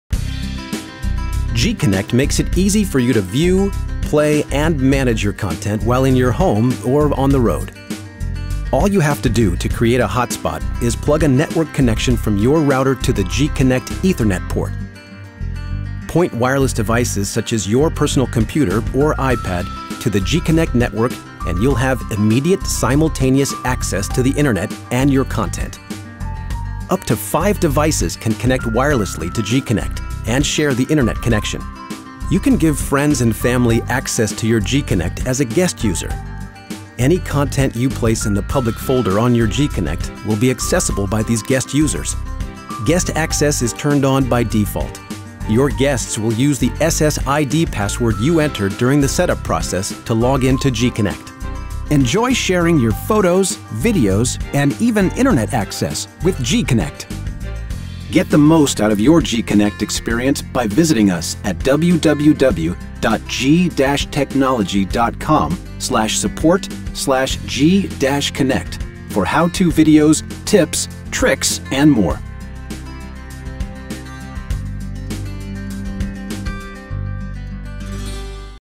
G-CONNECT, Wireless Hotspot
G-CONNECT can wirelessly stream high definition content to up to three devices or standard definition content to up to five devices simultaneously. It even acts as a WiFi Internet access point for five+ users!